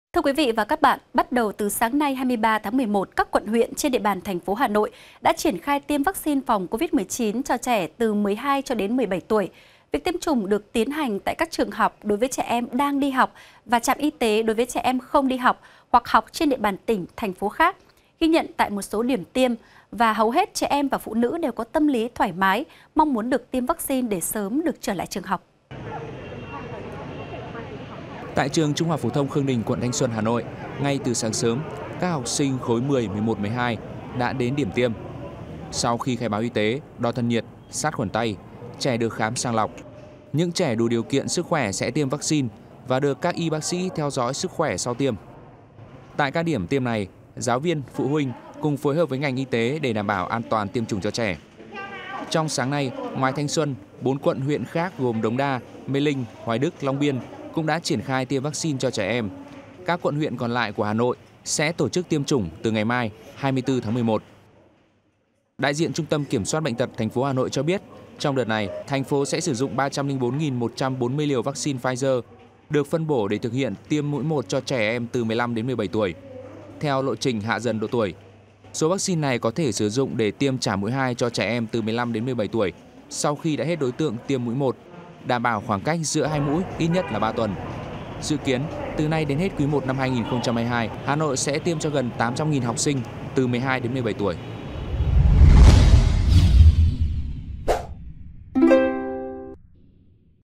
Hà Nội bắt đầu tiêm Vắc Xin Covid-19 cho Trẻ em| VTC14
VTC14 |Bắt đầu từ ngày 23/11, các quận, huyện trên địa bàn TP Hà Nội đã triển khai tiêm vắc xin phòng Covid-19 cho trẻ em từ 12-17 tuổi. Việc tiêm chủng được tiến hành tại các trường học đối với trẻ em đang đi học và tại trạm y tế với trẻ em không đi học hoặc học trên địa bàn tỉnh,thành phố khác. Tại 1 số điểm tiêm, hều hết trẻ và phụ huynh đều có tâm lý thoải mái, mong muốn được tiêm vắc xin để sớm được trở lại trường học.